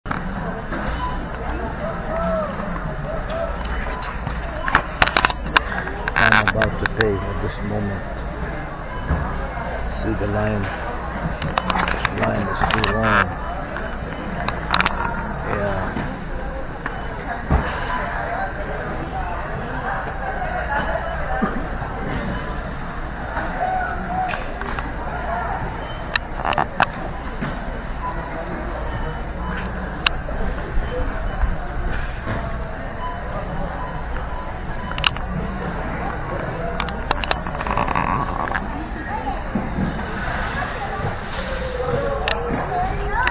FALL IN LINE TO PAY!
The system in paying is very organize and quick regardless how long is the line. Costco is one of the best store in "AMERICA"! But its annual membership went up.